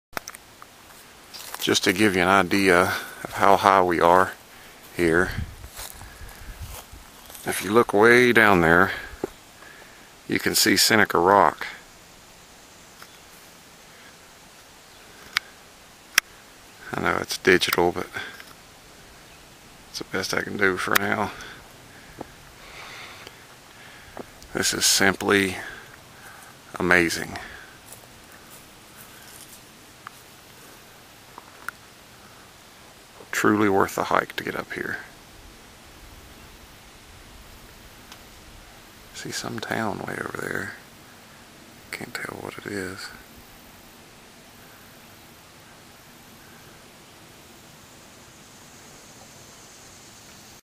Fall Foliage North Fork Mountain Seneca Rock
ForestWander Nature Photography
Amazing Fall Scenery from the top of North Fork Mountain Range. Looking below you can see Seneca Rock far below. When visiting Seneca Rock it seems very high. However, it seems small here!